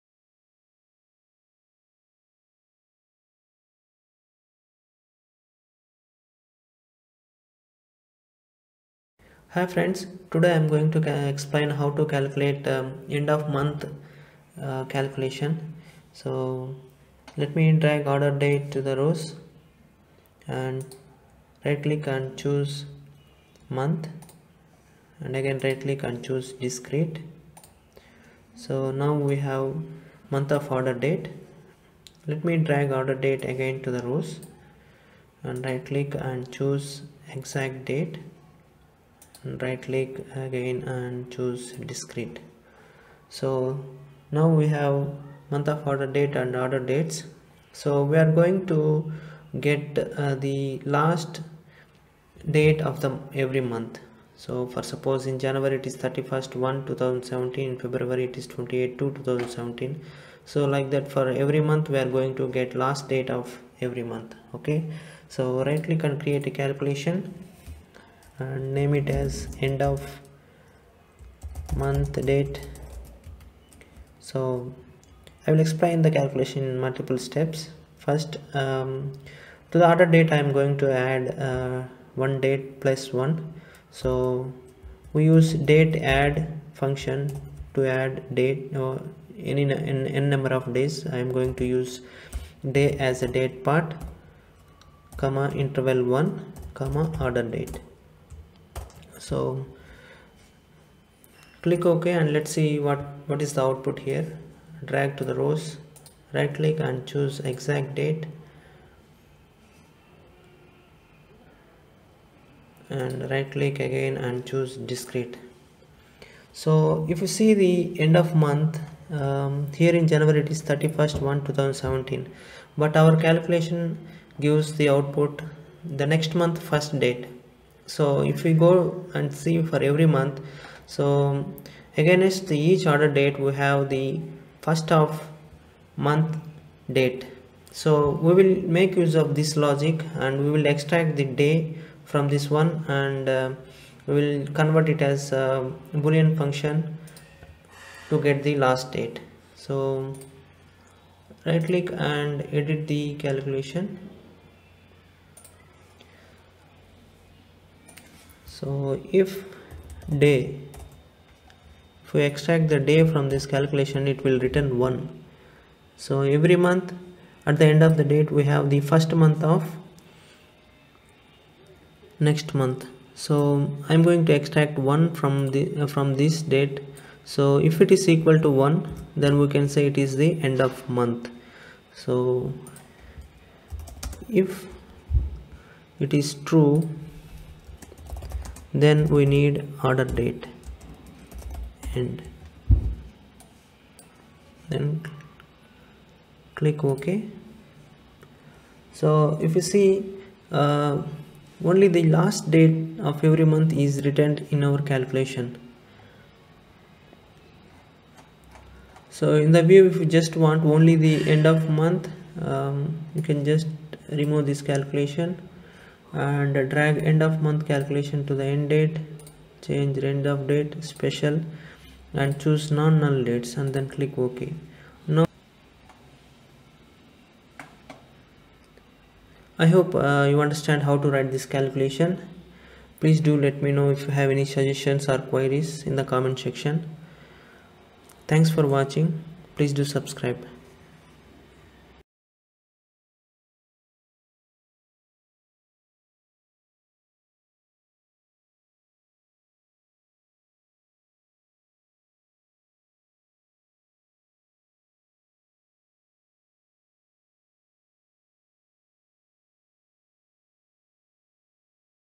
Tableau calculation to get end date of every month in Tableau?
I have explained how to write Tableau calculation to get end date of each month.
Functions used in this tutorial are - Dateadd() and Day()

Do let me your queries/suggestions in the comment section.